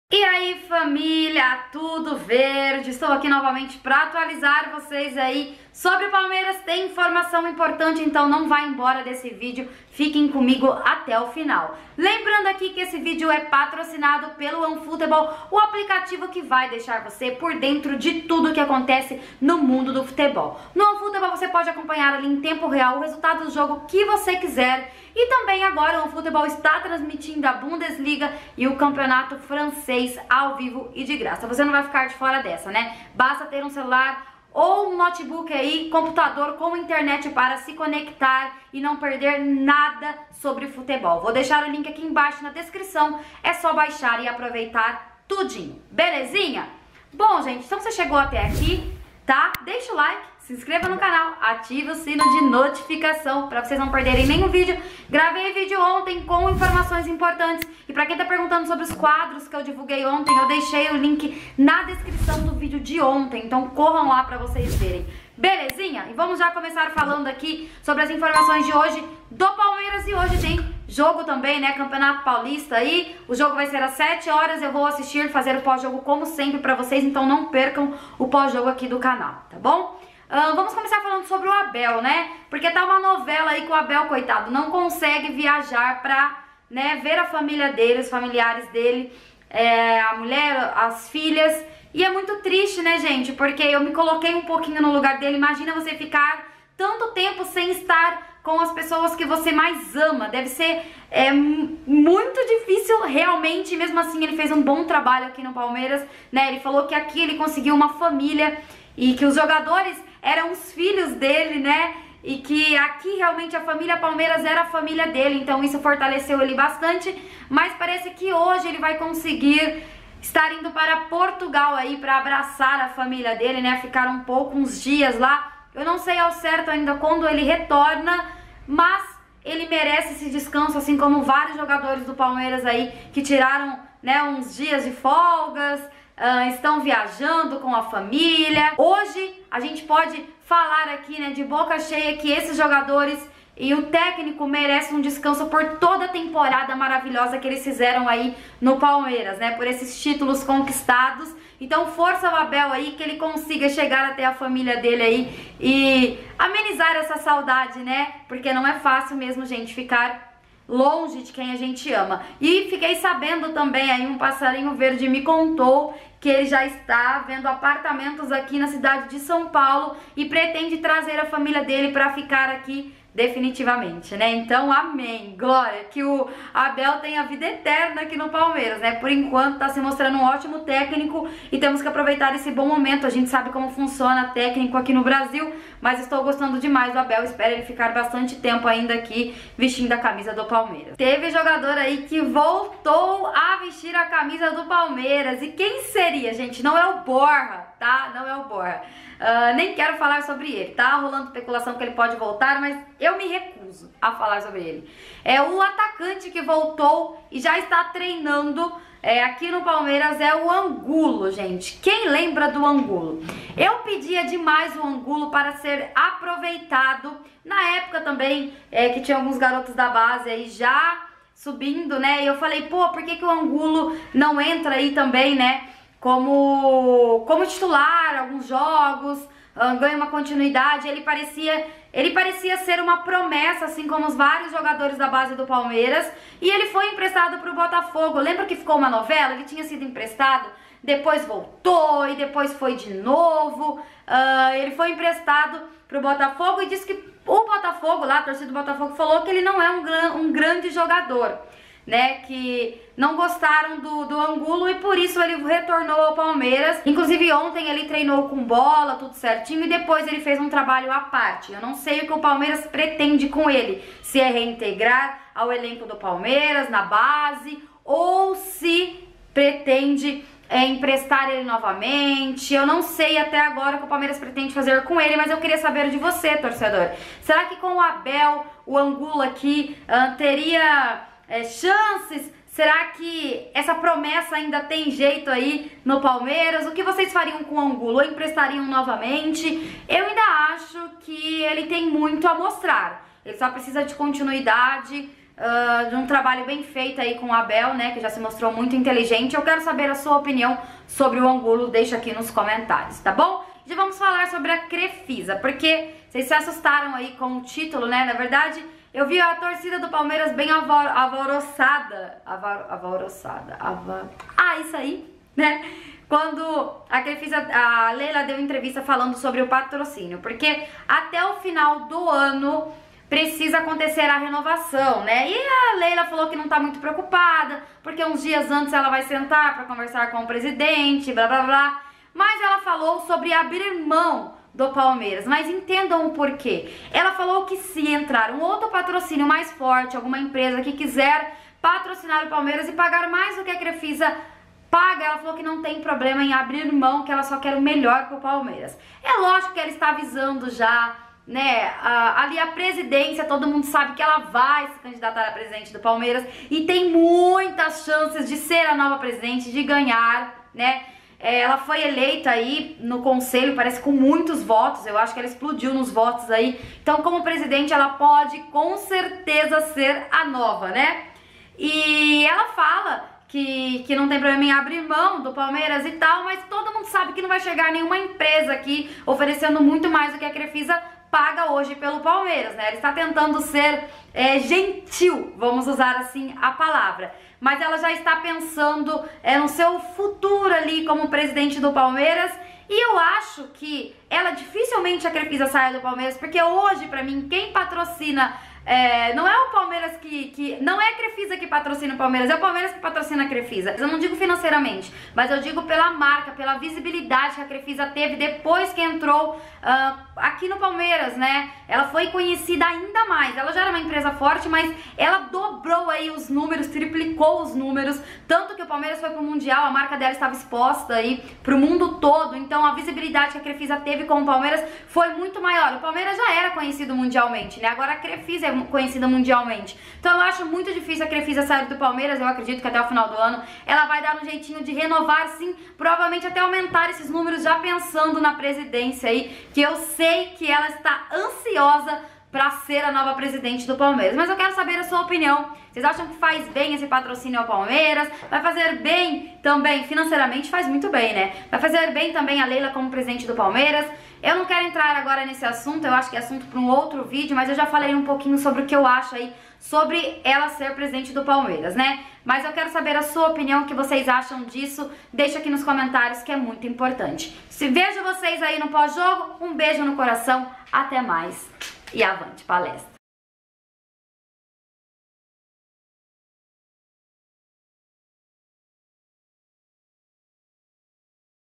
🚨 CREFISA VAI ABRIR MÃO DO PALMEIRAS? | ENTENDAM TUDO | OFICIAL VEJA QUEM VOLTOU | ABEL PARTIU
Conta Itaú :
Kassia Alves Poggi
AG: 2078
CONTA: 34889-9